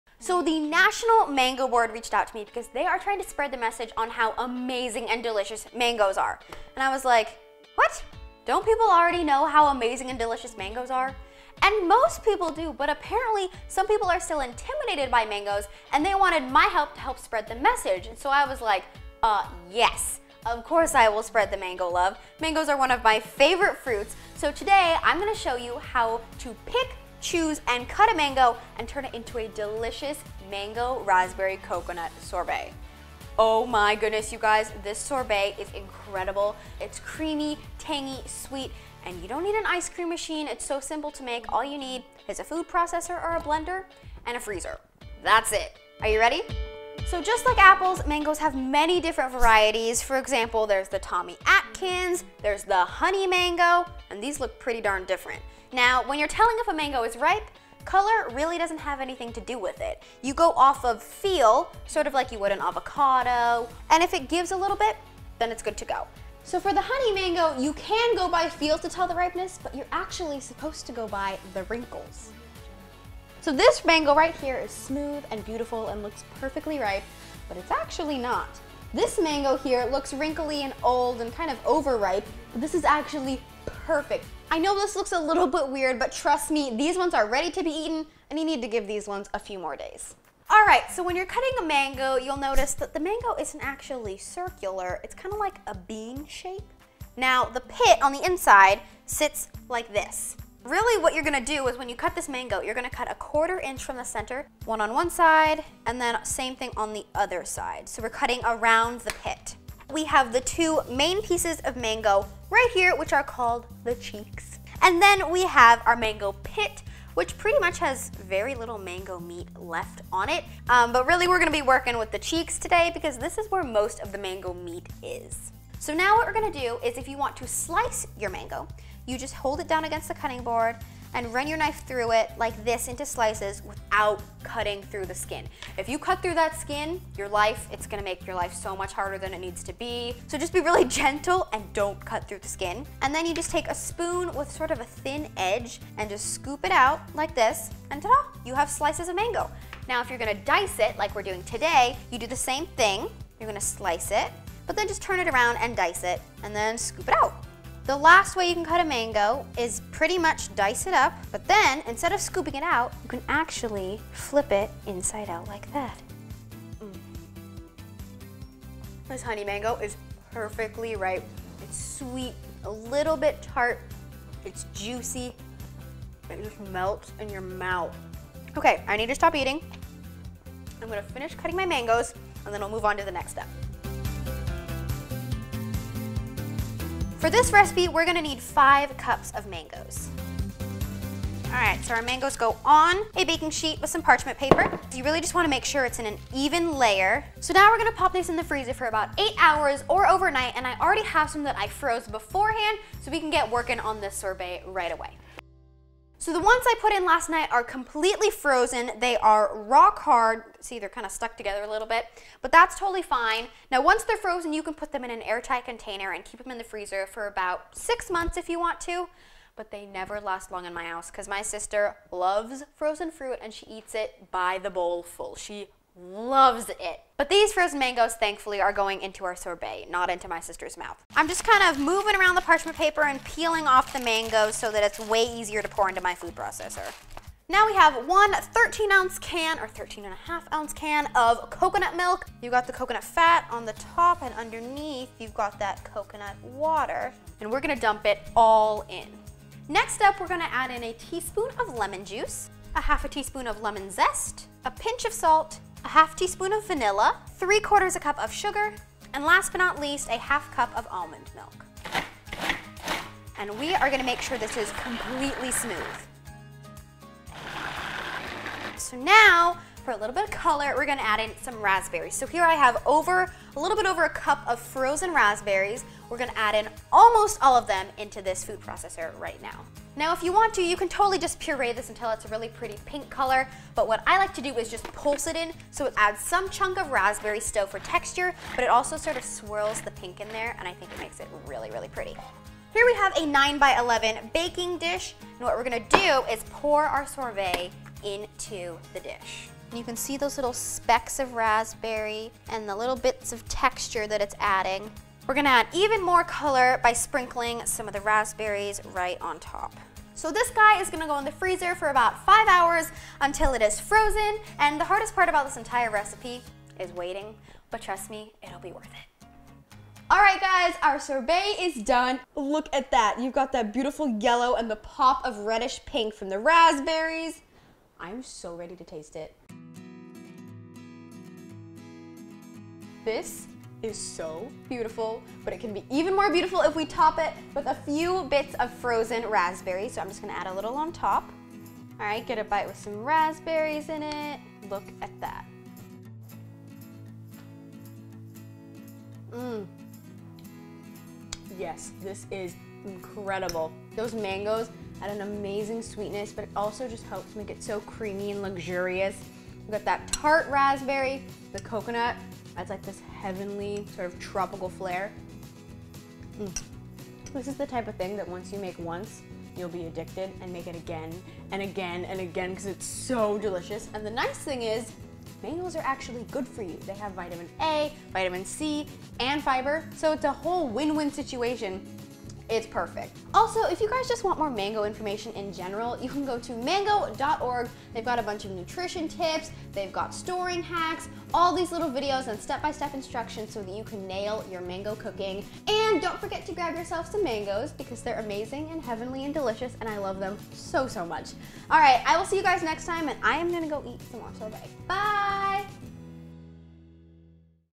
Mango 101 + Mango Raspberry Coconut Sorbet | Cook With Amber
This Mango Raspberry Coconut Sorbet is something that when you make once, you'll make over and over...it's SO DELICIOUS and easy to make! It really shows off the sweet mango, which is complimented by tart raspberries and the silky coconut milk. It's a great treat on a hot day, and you can even serve it as an elegant dessert!

There aren't many things more mouthwatering and delicious than a ripe, tender, sweet mango, But sometimes it can be hard to choose the right one. So, as we make the sorbet, I'll show you how to pick the PERFECT mango, and easy ways to cut it up perfectly! Mangos are one of my favorite fruits, and they are in season RIGHT NOW!

Thank you so so much to the National mango board for the mangos and for your amazing website that taught me so many cool mango facts and recipes.

- Amber